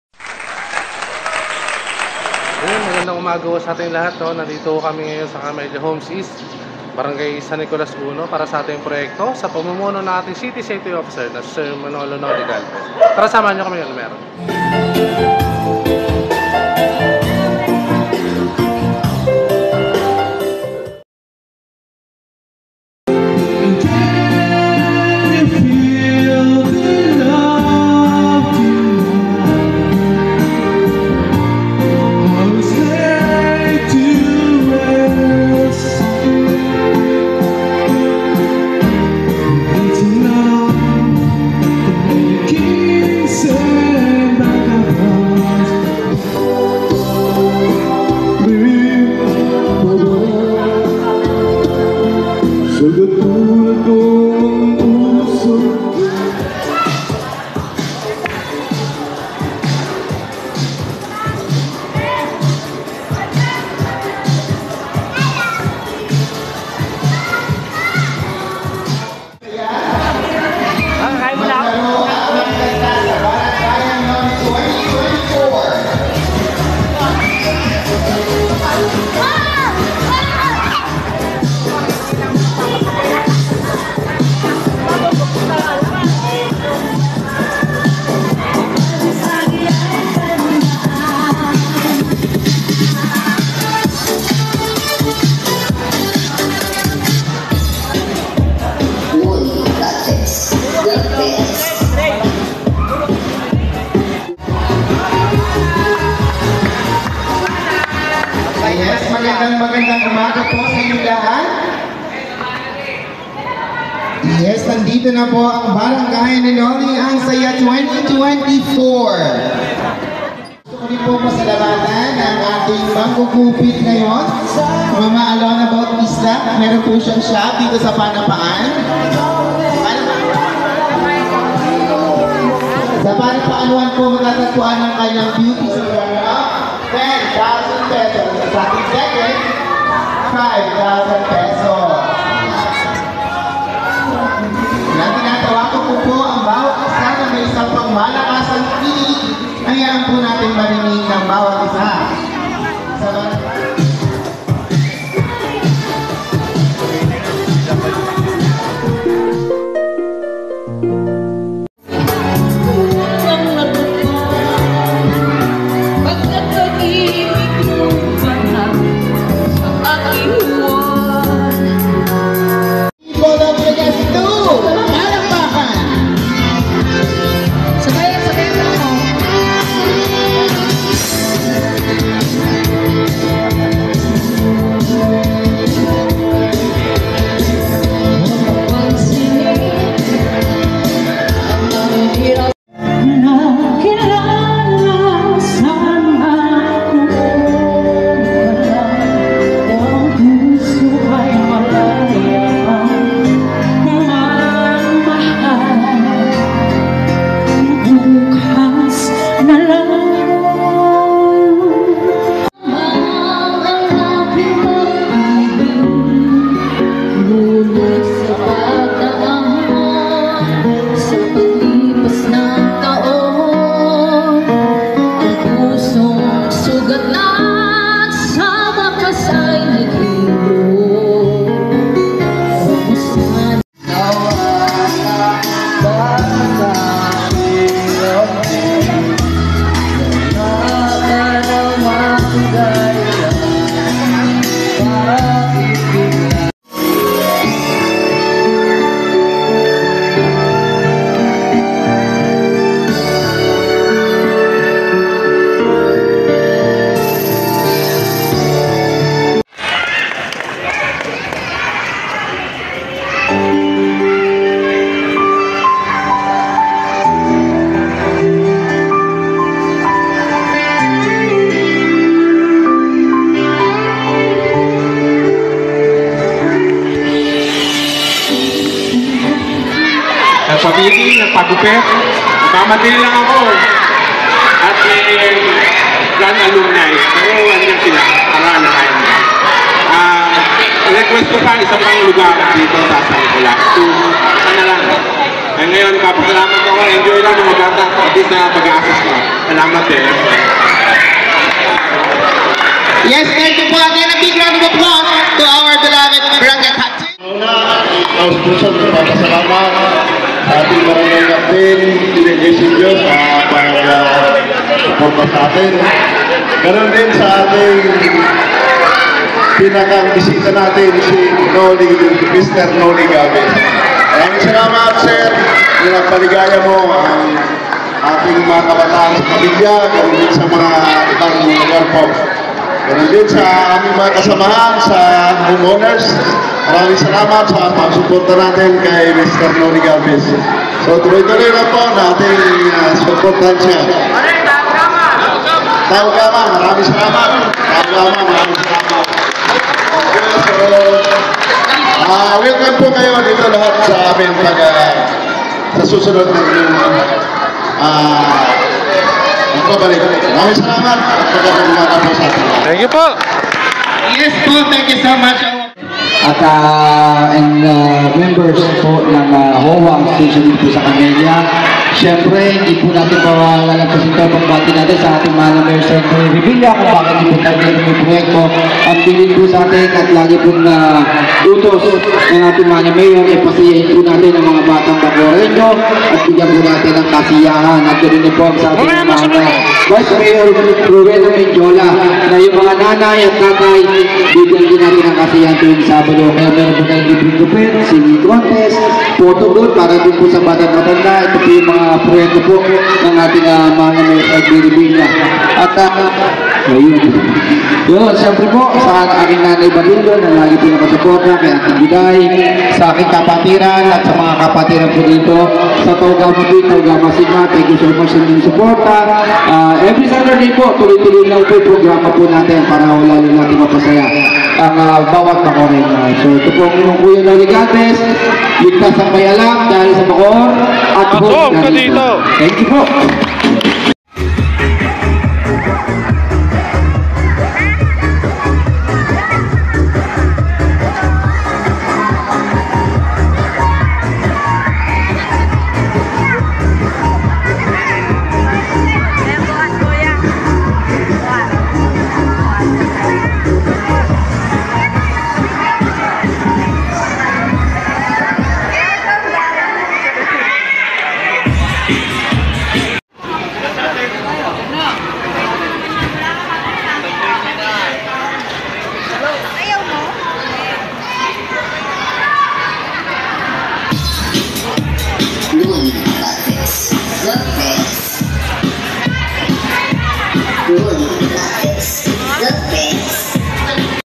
Weekly Project of TriGov Bacoor Chairman and Bacoor City Safety Officer Manolo "Noly" Galvez
Salamat din po sa TRIGOV BACOOR, Tau Gamma Phi Bacoor City Council at sa mga Chapters na tumulong.
Salute.